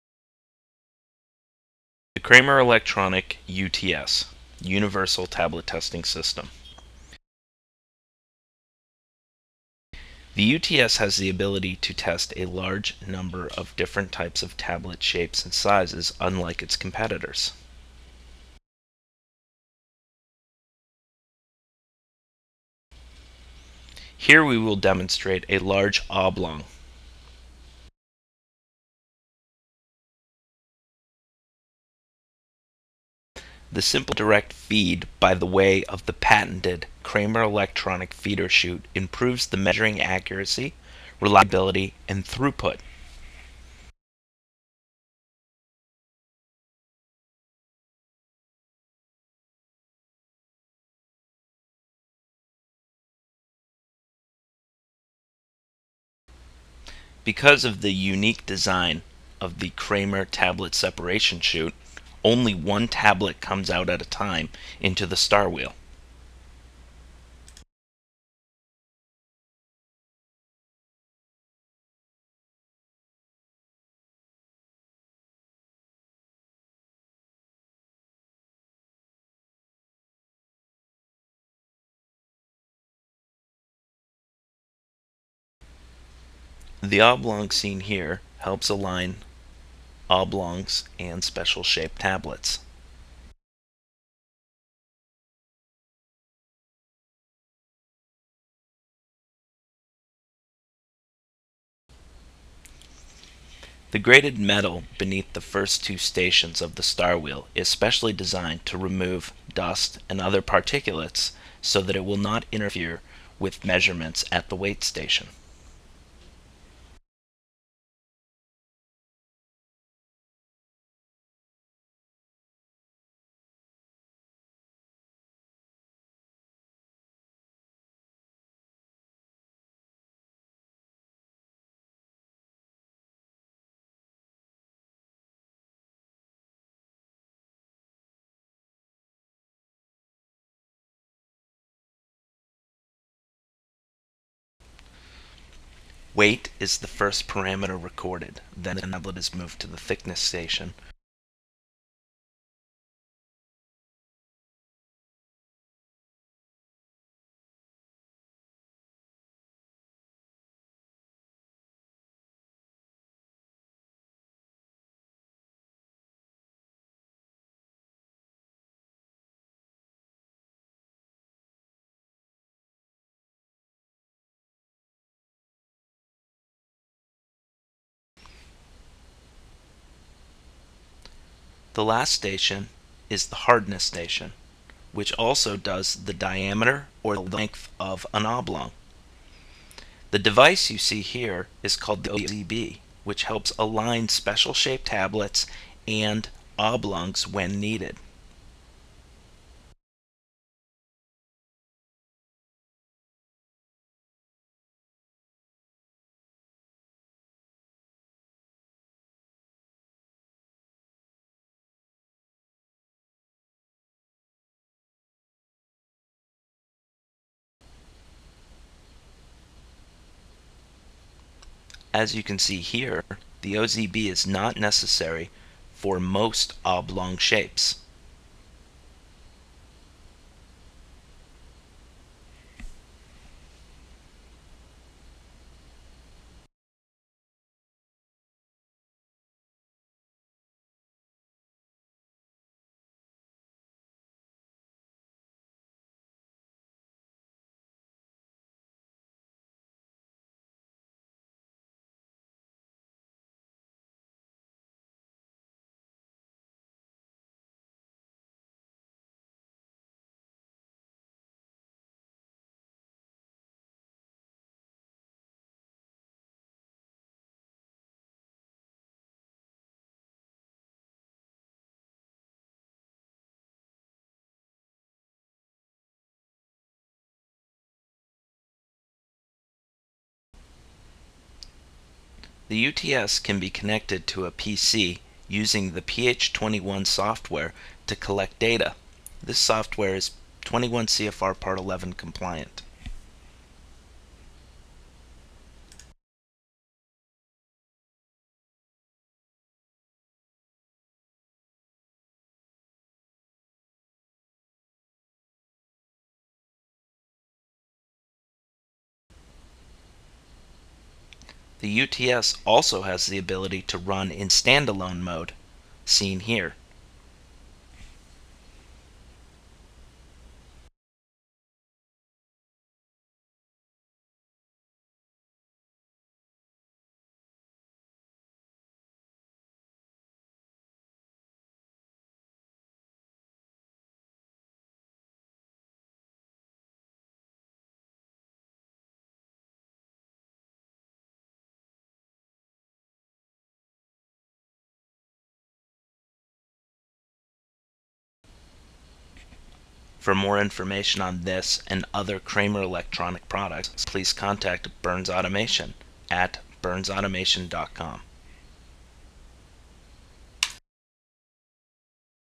Automatic Tablet Tester (Weight, Thickness, Diameter, Hardness)
The flexible and modular Kraemer Tablet Testing System UTS fully automatically measures weight, thickness, diameter and hardness of tablets, round, oblong tablets, capsules and similar products. This systme can be conncted with many presses including Korsch, Fette, and Ima.